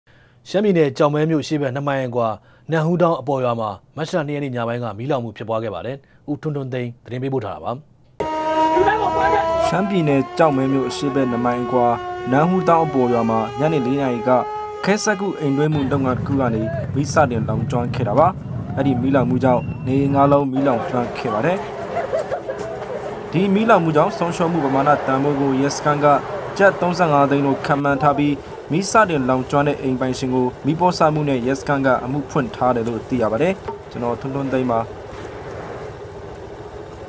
DVB - ေက်ာက္မဲၿမဳိ႔ အေရွ႔ဘက္မွာ မီးေလာင္မႈ ျဖစ္ပြားခဲ့
ရွမ္းျပည္နယ္ ေက်ာက္မဲၿမိဳ႕ အေရွ႕ဘက္ ၂ မိုင္အကြာ နမ့္ဟူးေတာင္းအေပၚရြာမွာ မတ္ ၂ ရက္ ညေနပိုင္းက မီးေလာင္မႈ ျဖစ္ပြြားခဲ့ပါတယ္။ ဦးထြန္းထြန္းသိန္း သတင္းေပးပိုု႔ ထားပါတယ္။ DVB TV - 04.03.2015